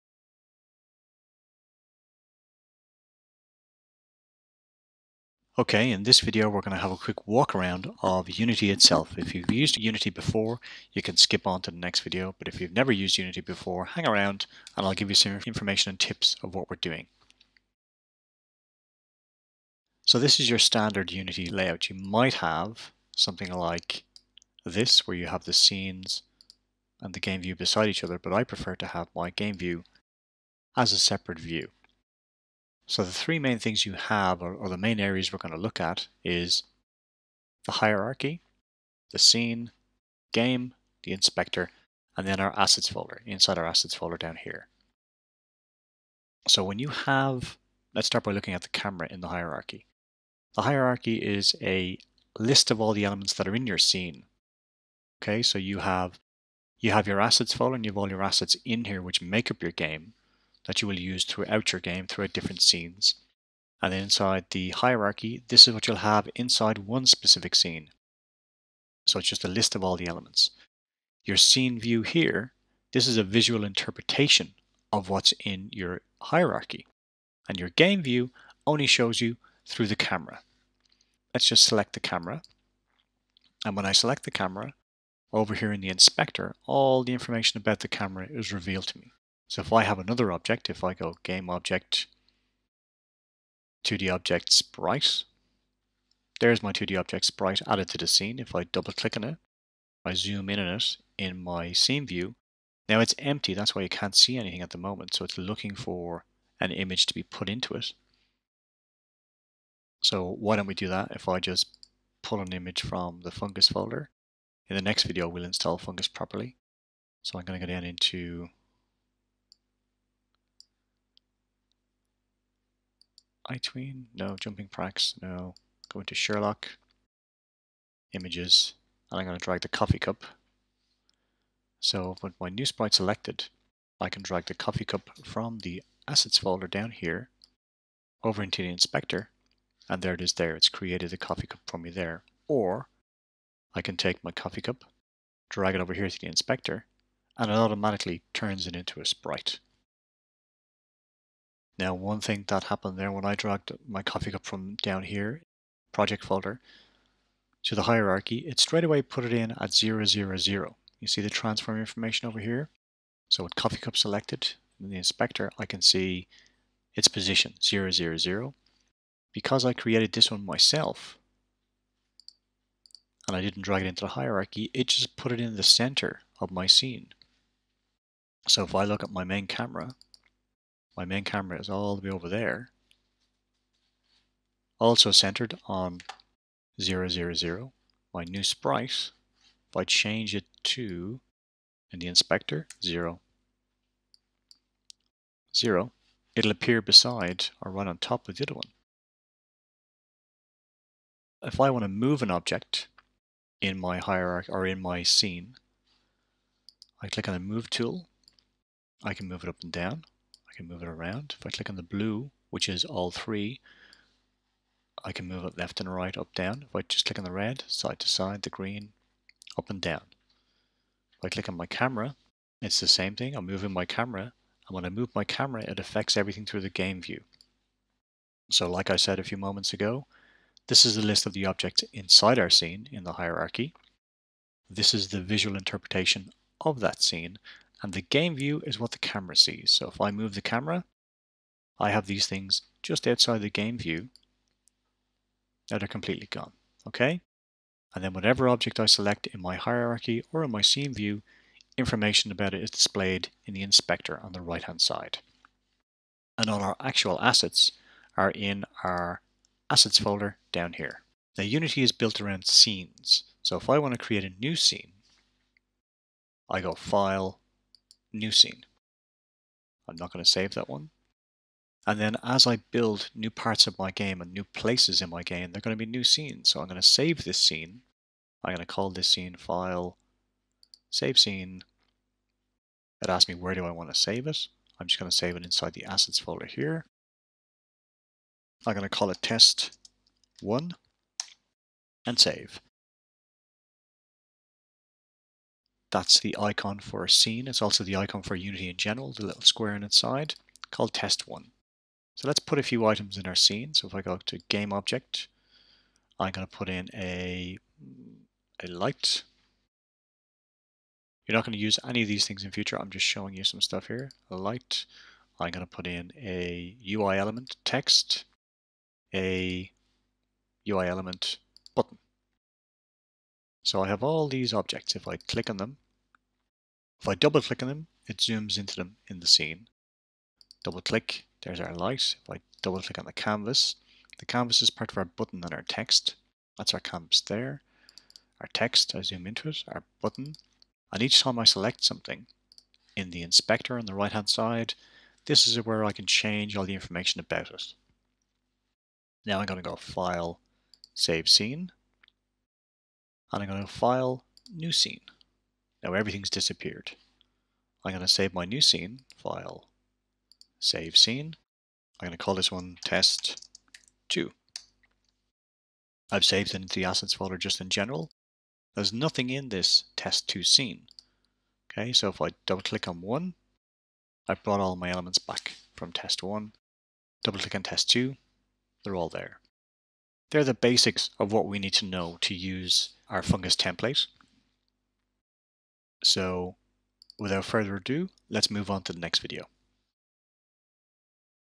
Create Puzzle Games with unity 3D & Fungus
Create Puzzle Games with unity 3D & Fungus -   join me as i take a look at how to use unity and fungus to create a basic adventure game...